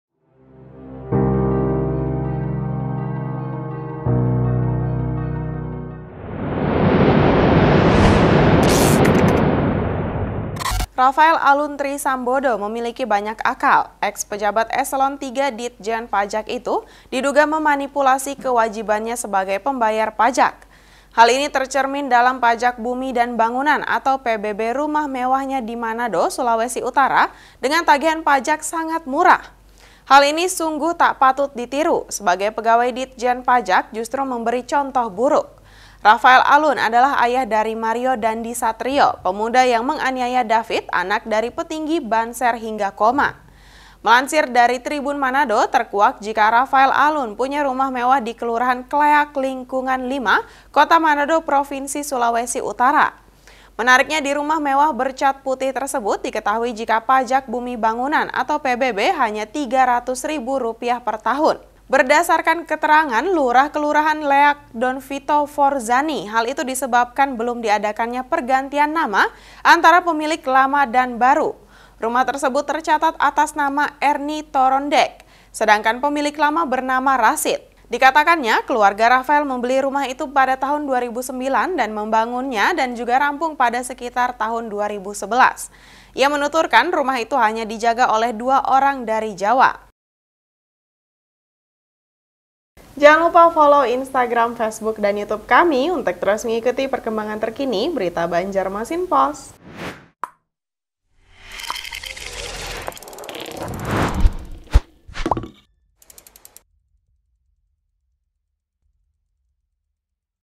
Rafael Alun Diduga Manipulasi Pembayaran Pajak Rumah Mewahnya, PBB Hanya Rp 300 Ribu
BANJARMASINPOST.CO.ID - Rafael Alun Trisambodo memiliki banyak akal. Eks pejabat eselon III Ditjen Pajak itu diduga memanipulasi kewajibannya sebagai pembayar pajak.

Hal ini tercermin dari Pajak Bumi dan Bangunan rumah mewahnya di Manado, Sulawesi Utara, dengan tagihan pajak sangat murah.

Hal ini sungguh tak patut ditiru, sebagai pegawai Ditjen Pajak justru memberi contoh buruk.

Fakta-fakta tentang harta Rafael pun terus bermunculan, salah satunya adalah rumah mewah di Manado.

Melansir dari Tribunmanado, terkuak jika Rafael Alun punya rumah mewah di Kelurahan Kleak, Lingkungan 5, Kota Manado, Provinsi Sulawesi Utara.

Menariknya di rumah mewah bercat putih tersebut diketahui jika PBB hanya Rp 300 ribu pertahun.

Berdasarkan keterangan, Lurah Kelurahan Kleak Donvito Fourzany hal itu disebabkan belum diadakannya pergantian nama antara pemilik lama dan baru.

"Akibatnya pemilik baru membayar dengan nilai sesuai ukuran rumah lama," katanya, Selasa (28/2/2023).

Ia menuturkan, rumah tersebut tercatat atas nama Ernie Torondek.

Sedang pemilik lama bernama Rasid. Dikatakannya, keluarga Rafael membeli rumah itu 2009, membangunnya dan rampung pada sekira 2011.

Ia menuturkan, rumah itu hanya dijaga oleh dua orang dari Jawa.(Tribun-video.com)